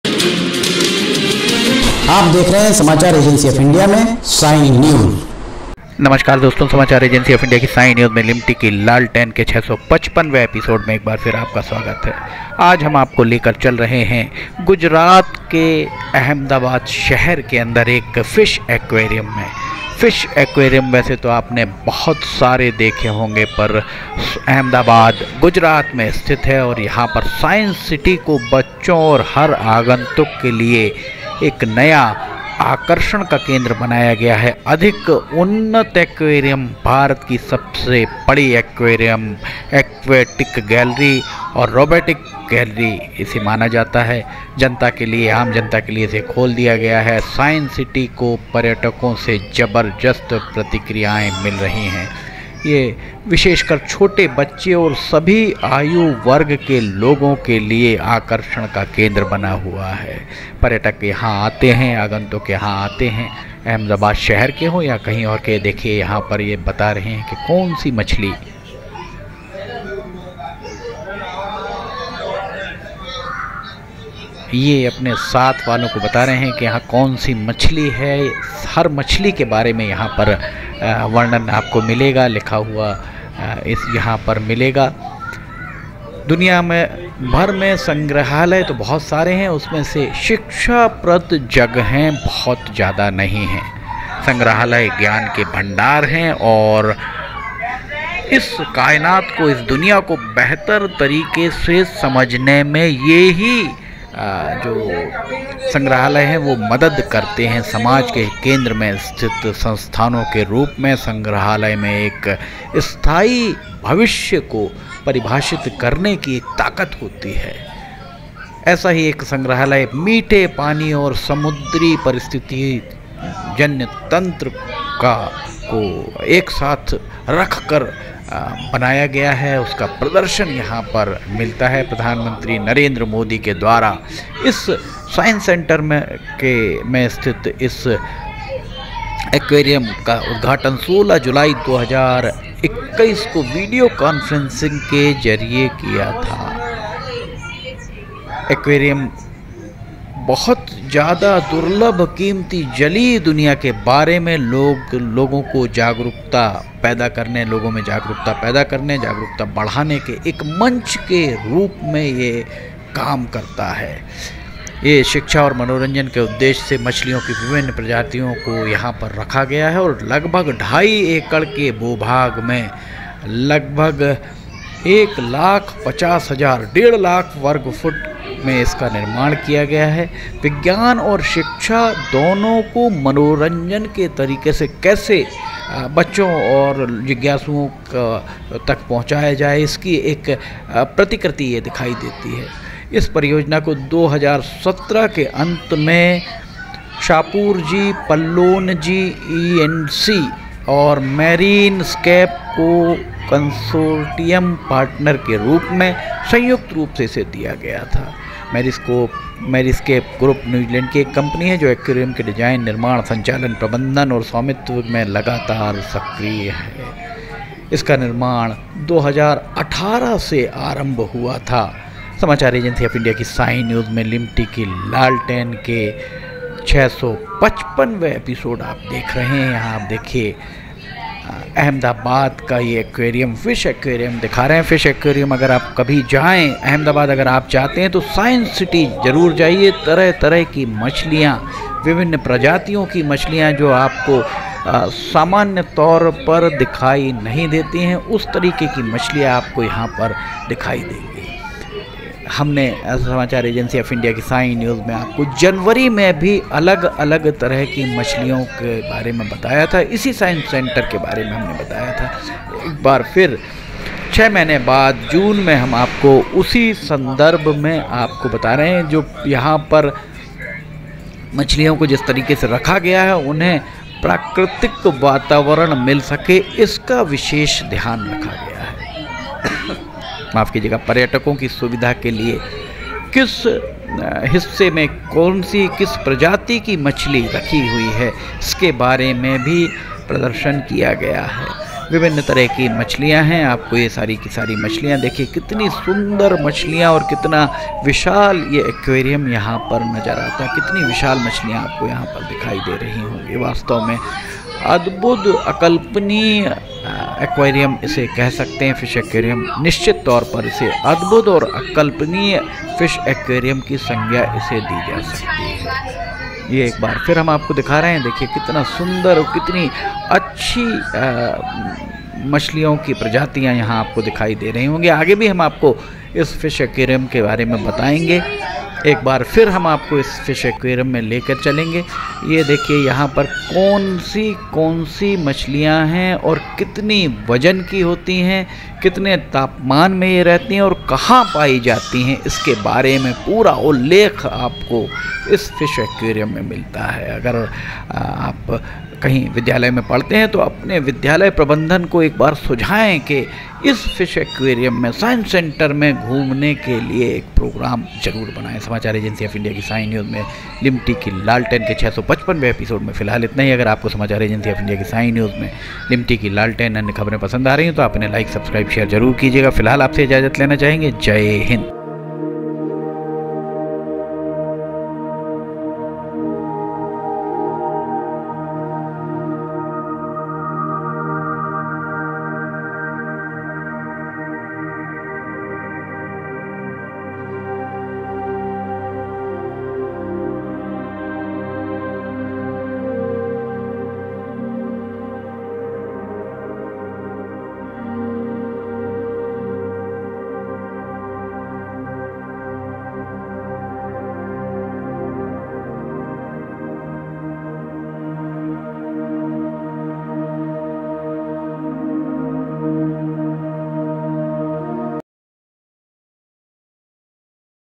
मनोरंजन और विज्ञान के हिसाब से बहुत ही उत्तम है अहमदाबाद के साईंस सेंटर स्थित यह FISH AQUARIUM
गुजरात के अहमदाबाद जाएं तो साईंस सैंटर का भ्रमण जरूर करें, विज्ञान को करीब से देखने का मिलेगा मौका